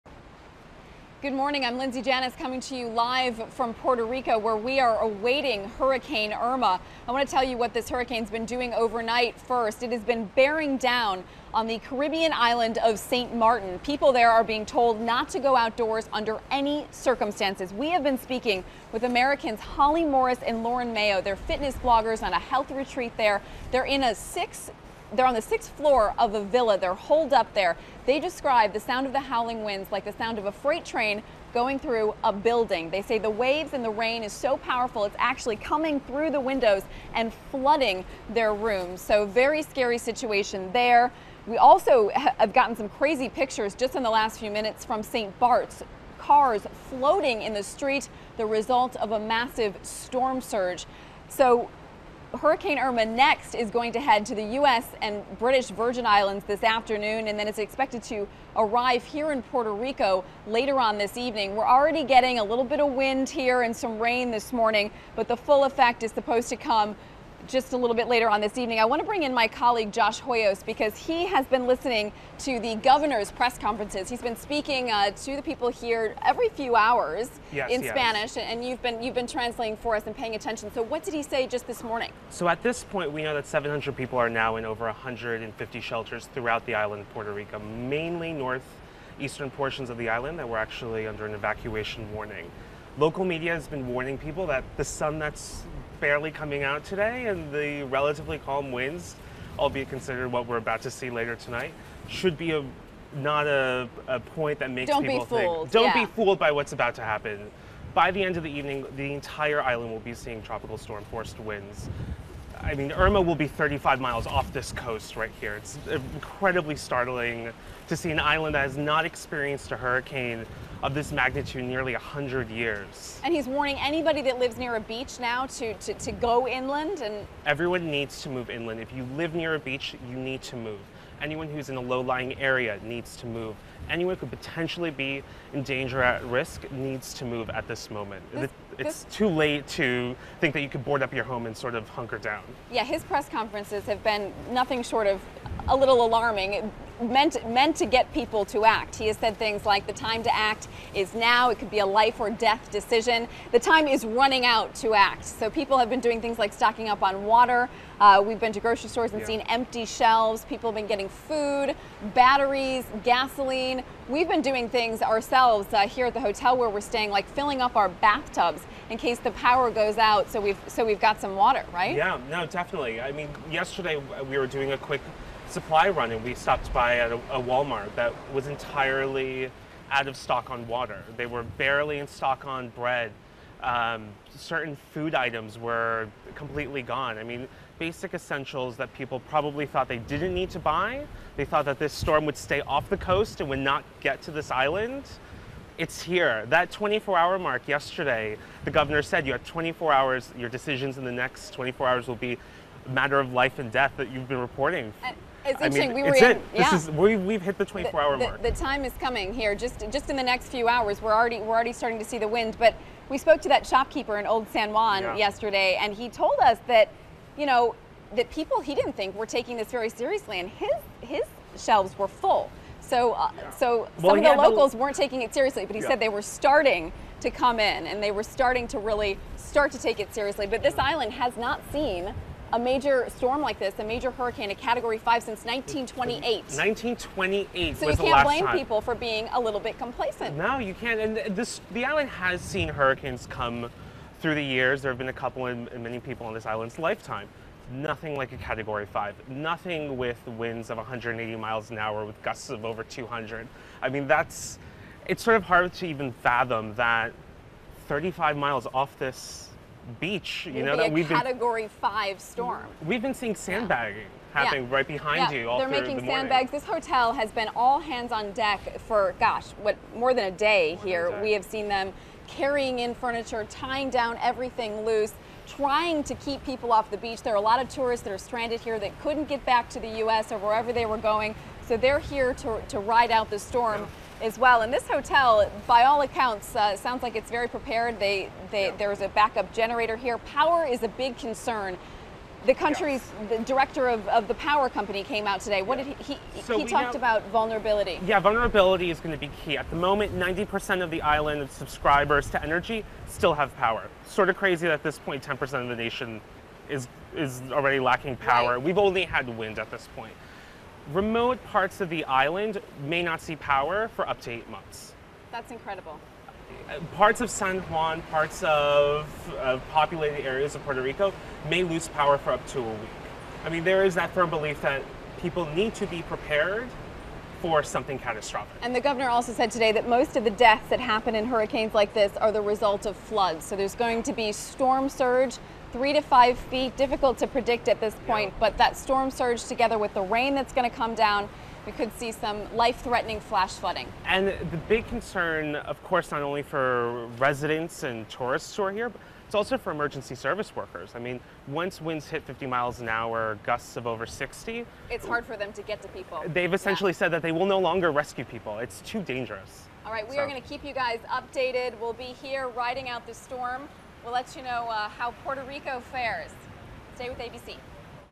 Hurricane Irma Hits Caribbean as Florida, Georgia, SC
Hurricane Irma lashed the Caribbean today, causing at least three deaths, while farther north in the U.S., Florida, Georgia and South Carolina are facing states of emergency as residents brace for the monster storm.

The storm -- dubbed "extremely dangerous" by the National Hurricane Center (NHC) currently has sustained winds of 185 mph and even higher gusts.

The storm's impact on the Caribbean islands

On Wednesday afternoon, Irma was battering the Caribbean, destroying about 90 percent of the structures and vehicles on Barbuda, officials said.

In St. Martin and St. Barthelemy, at least two people died and at least two others were seriously wounded, according to a spokesperson for France’s ministry for its overseas territories.

At least one person died in Barbuda, where there is widespread damage, said Midcie Francis, a spokeswoman for Antigua & Barbuda’s National Office of Disaster Services.

 Irma, which is approximately 450 miles wide, has had sustained winds of at least 180 mph for a longer period of time than any other Atlantic storm on record.

As of 5 p.m. ET, Irma was about 55 miles east of San Juan, Puerto Rico and moving west at about 16 mph. The hurricane is forecast to move north of U.S. territory this evening, unleashing strong winds and heavy rain.

Persistent heavy rain from Irma is expected to continue for hours and flash flood warnings have been issued across northeastern Puerto Rico, including San Juan.

Hurricane warnings have been issued for much of the central Bahamas, while a hurricane watch has been issued for much of the northwestern Bahamas.

Tropical storm warnings have been issued for parts of Cuba, including Guantanamo, Holguin, and Las Tunas provinces. The hurricane warning for Saint Martin has been discontinued.

The track of Hurricane Irma

The storm will gradually begin to weaken as it crosses through the Caribbean, but it will still remain a powerful Category 5 storm through Friday, forecasters predict.

Irma's projected path goes through the Bahamas Friday into Saturday morning and the storm is expected to curve north toward Florida by late Saturday afternoon.

On Sunday morning, Irma is expected to approach just south of Miami as a Category 4 hurricane with winds of 145 mph. Forecasts show Irma weakening to a Category 3 hurricane near Daytona Beach on Monday morning as it creeps up the east coast.

The storm is expected to then move up the Florida coast as a Category 3 hurricane with winds of 125 mph.

Georgia and South Carolina are in the northern edge of the forecast cone as well.
 The current spaghetti models show Irma taking an eastern shift, which could mean a possible landfall on the coasts of Georgia and South Carolina.

Some spaghetti models show the storm moving even further away from Florida's east coast.

People are stranded on Caribbean islands

Today, as the rain pummels Puerto Rico, some tourists and residents are stranded.

Rows of shops are boarded up today in one San Juan neighborhood as heavy wind and rain batters the area. The streets are deserted, with just a few scattered cars on the road.

 Puerto Rico Governor Ricardo Rossello warned of Irma's destructive and told the public, "There is no reason to be outside."

Officials said over 1,000 people are in shelters and some 300,000 people have lost power.

Two visitors to Puerto Rico who were holed up on the sixth floor of a villa told ABC News that the howling winds sound like a freight train going through a building. They said the rains and waves are so powerful that water is coming through windows and flooding their room.

A shopkeeper in Old San Juan told ABC News on Tuesday that his shelves were mostly full, and to him, it didn't seem as if all locals appeared to be taking the storm very seriously.

The Sunshine State prepares 'for the worst'

Preparations are already underway in Florida, which could face "direct impacts," according to the NHC, though it's too soon to tell for sure.

Florida Gov. Rick Scott -- who declared a statewide state of emergency on Monday -- said this afternoon about 25,000 people have already evacuated.

Over 200,000 Floridians have been ordered to evacuate: Mandatory evacuations have already been ordered for Monroe County, which includes the Florida Keys, and coastal parts of Broward County and Miami-Dade County.

 Many residents of the Keys -- which the governor said is expected to be affected on Sunday -- didn't wait until today's mandatory evacuation and instead evacuated Tuesday.

The Keys' famous tourist street, Duval Street, was nearly empty today.
Hurricane Irma threatens Florida
One Keys apartment building owner told ABC News she thinks the devastation left behind in the Houston area by Hurricane Harvey is on people's minds. She said nearly all of her tenants have evacuated, and she's encouraging them to do so, explaining that "this storm is different."